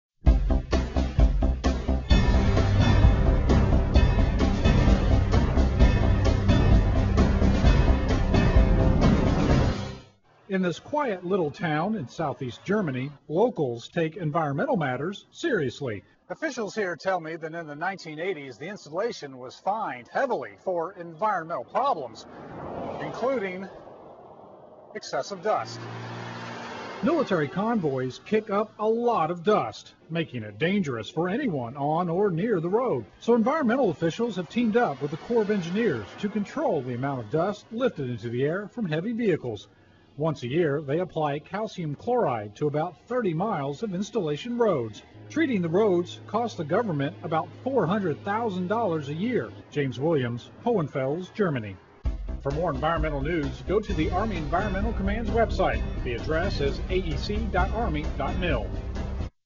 Dust Control
Army.mil - Environmental officials at Hohenfels, Germany, take measures to ensure the safety of Soldiers and civilians while managing the environment. James Williams has more.